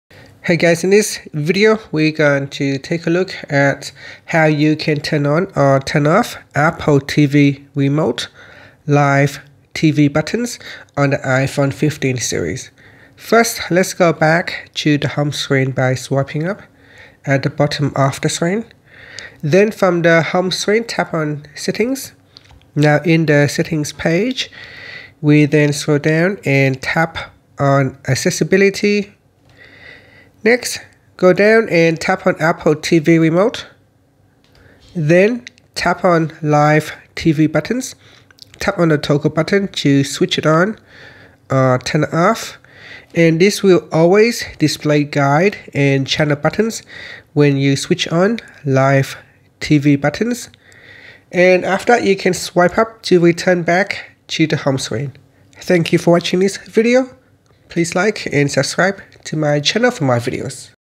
iPhone 15/15 Pro Max: How to Turn On/Off Apple TV Remote Live TV Buttons
Unlock the full potential of your iPhone 15/15 Pro Max with iOS 17! In this step-by-step tutorial, discover how to seamlessly control your Apple TV using the Live TV Buttons on your iPhone. 📺✨ Learn how to turn them on/off effortlessly and enhance your entertainment experience. Whether you're a tech enthusiast or a casual user, this guide is tailored for you! 💡